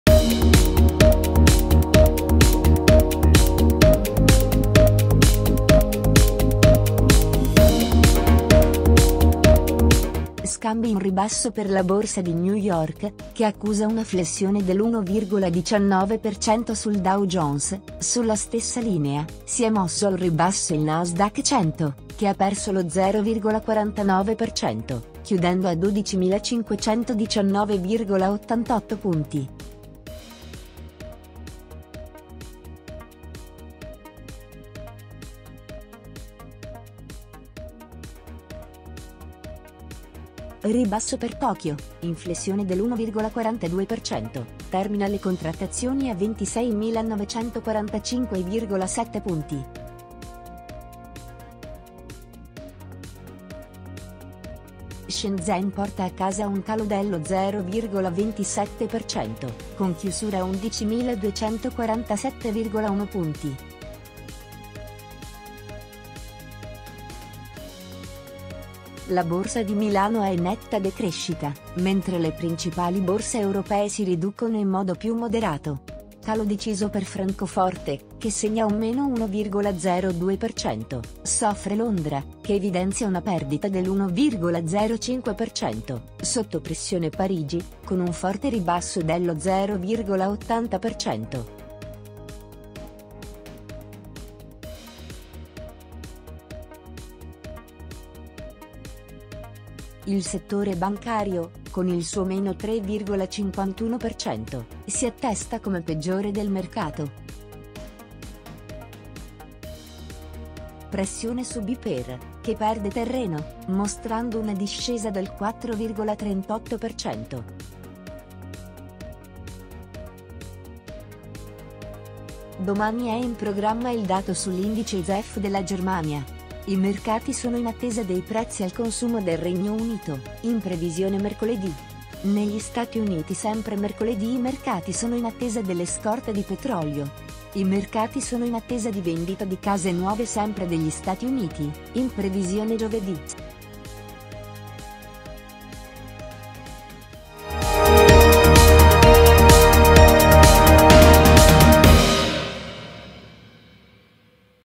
Buongiorno dalla Borsa 20 marzo 2023
Scambi in ribasso per la Borsa di New York, che accusa una flessione dell'1,19% sul Dow Jones; sulla stessa linea, si è mosso al ribasso il Nasdaq 100, che ha perso lo 0,49%, chiudendo a 12.519,88 punti.

Ribasso per Tokyo, in flessione dell'1,42%, termina le contrattazioni a 26.945,7 punti. Shenzhen porta a casa un calo dello 0,27%, con chiusura a 11.247,1 punti.

La Borsa di Milano è in netta decrescita, mentre le principali Borse europee si riducono in modo più moderato. Calo deciso per Francoforte, che segna un -1,02%; soffre Londra, che evidenzia una perdita dell'1,05%; sotto pressione Parigi, con un forte ribasso dello 0,80%.

Il settore bancario, con il suo -3,51%, si attesta come peggiore del mercato. Pressione su BPER, che perde terreno, mostrando una discesa del 4,38%.

Domani è in programma il dato sull'Indice ZEW della Germania. I mercati sono in attesa dei Prezzi al Consumo del Regno Unito, in previsione mercoledì. Negli Stati Uniti sempre mercoledì i mercati sono in attesa delle Scorte di Petrolio. I mercati sono in attesa di Vendita di Case Nuove sempre degli Stati Uniti, in previsione giovedì.

Sui social